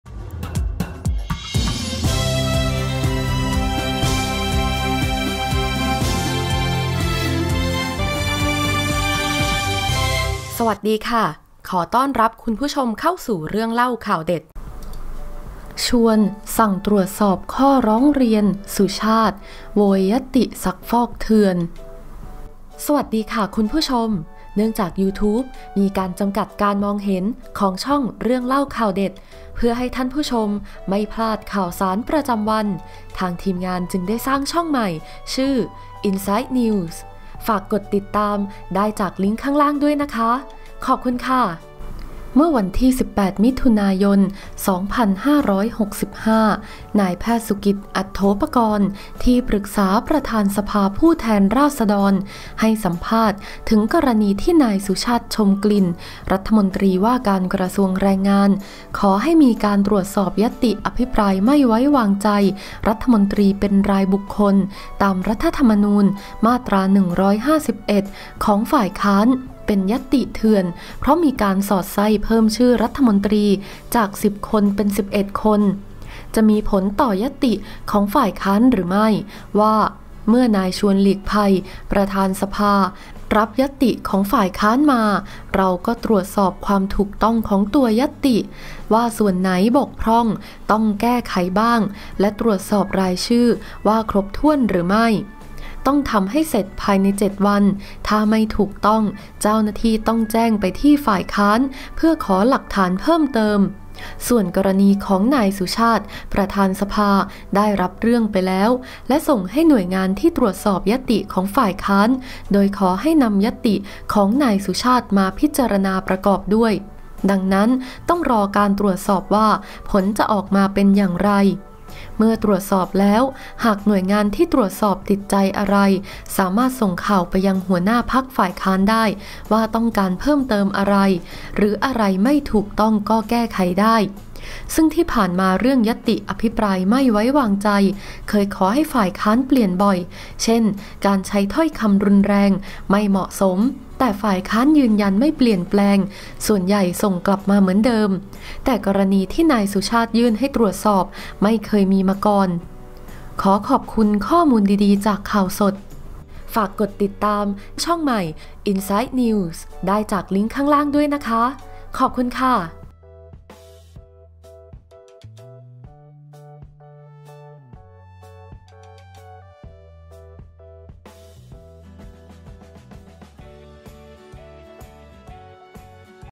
เอาที่สบายใจ! "ชวน" สั่งตรวจสอบข้อร้องเรียน ‘สุชาติ’ หลังดิ้นพล่าน โวยญัตติซักฟอกเถื่อน
ติดตามช่อง insight NEWS กดลิงค์ด้านล่างเพื่อรับชม

นพ.สุกิจ อัถโถปกรณ์ ที่ปรึกษาประธานสภาผู้แทนราษฎร ให้สัมภาษณ์ถึงกรณีที่นายสุชาติ ชมกลิ่น รมว.แรงงาน พรรคพลังประชารัฐ ขอให้มีการตรวจสอบญัตติ อภิปรายไม่ไว้วางใจรัฐมนตรีเป็นรายบุคคล ตามรัฐธรรมนูญ มาตรา 151 ของฝ่ายค้าน เป็นญัตติเถื่อน เพราะมีการสอดไส้เพิ่มชื่อรัฐมนตรีจาก 10 คน เป็น 11 คน จะมีผลต่อญัตติของฝ่ายค้านหรือไม่ ว่า เมื่อนายชวน หลีกภัย ประธานสภา พรรคประชาธิปัตย์ รับญัตติของฝ่ายค้านมา เราก็ตรวจสอบความถูกต้องของตัวญัตติ ว่ามีส่วนไหนบกพร่องต้องแก้ไขบ้าง และตรวจสอบรายชื่อว่าครบถ้วนหรือไม่ ต้องทำให้เสร็จภายใน 7 วัน ถ้าไม่ถูกต้องเจ้าหน้าที่ต้องแจ้งไปที่ฝ่ายค้าน เพื่อขอหลักฐานเพิ่มเติม
ส่วนกรณีของนายสุชาติ ประธานสภาได้รับเรื่องไปแล้ว และส่งให้หน่วยงานที่ตรวจสอบญัตติของฝ่ายค้าน โดยขอให้นำญัตติของนายสุชาติมาพิจารณาประกอบด้วย ดังนั้นต้องรอการตรวจสอบว่าผลจะออกมาเป็นอย่างไร